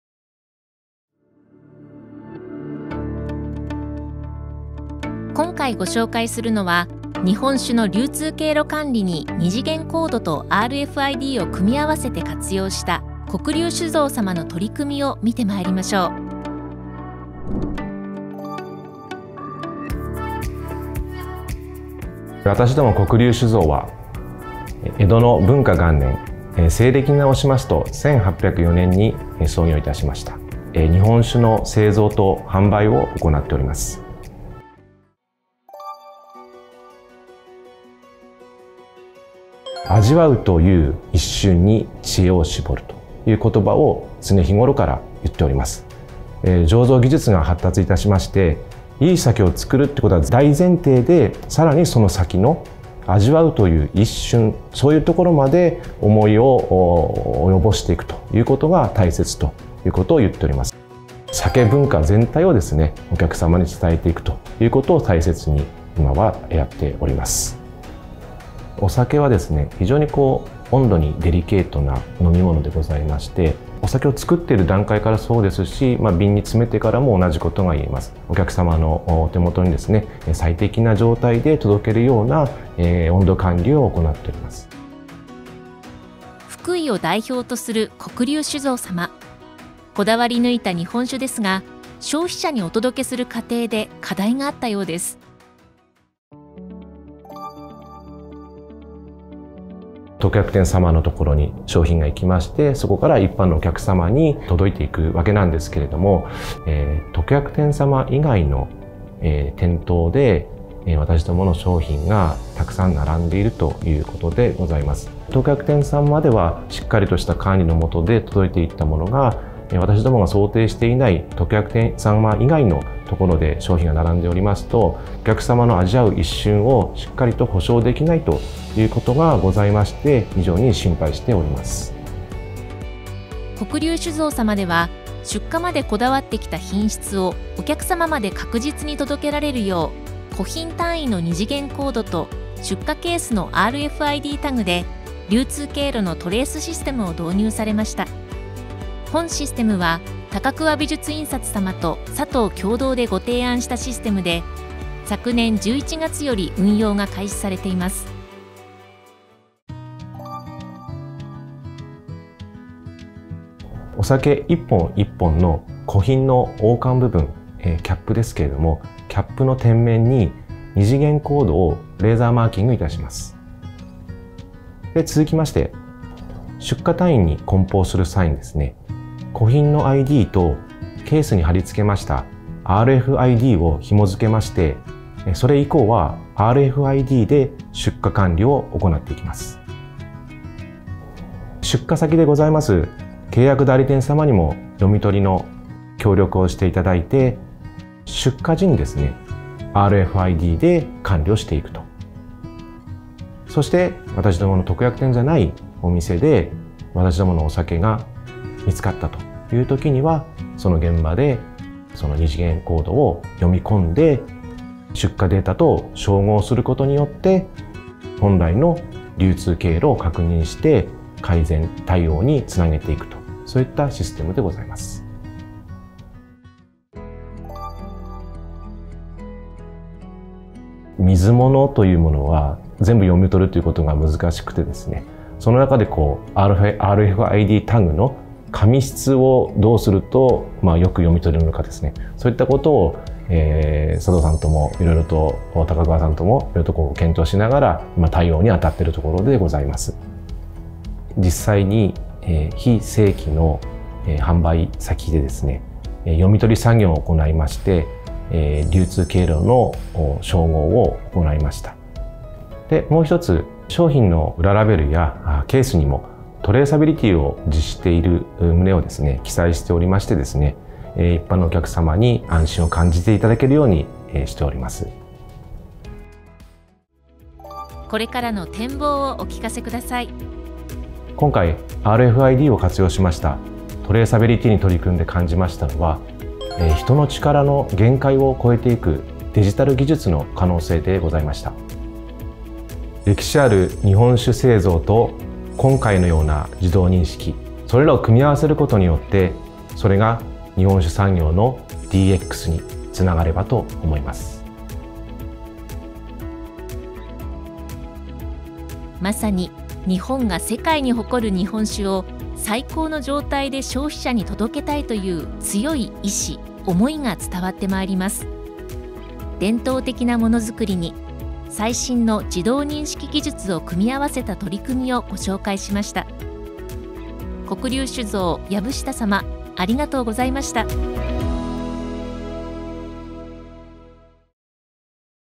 日本酒の正規品流通経路管理のための、2次元コードとRFIDハイブリッド活用｜黒龍酒造様導入事例
このシステムは日本酒の商品一本ごとの流通経路を把握し、並行品の流通拡大を抑止することで、より安心な商品の提供を目指すものです。2次元コードとRFIDタグを組み合わせて、酒造メーカーから買い入れる卸売業者または特約酒販店、卸売業者から直接買い入れる酒販店までの出荷商品、出荷履歴を管理します。自動認識技術による情報一括読み取りなどにより、製造現場や出荷管理業務の負担も軽減します。

0:00 黒龍酒造様について
1:00 日本酒の温度管理へのこだわり
1:25 温度管理の課題
2:13 導入ソリューション
3:47 導入効果
4:37 自動認識技術への期待

商品の製造段階で、王冠の天面に2次元コードを印字して、個品での管理情報を付加します。
商品をケースに入れ、ケース内の商品情報をカメラで一括読み取り。ケース毎にRFIDタグ（シール）を発行し、ロボットでケースに自動で貼り付けます。
出荷の際にRFIDタグを一括読み取りして、データベースに出荷情報を入力し、流通経路を管理します。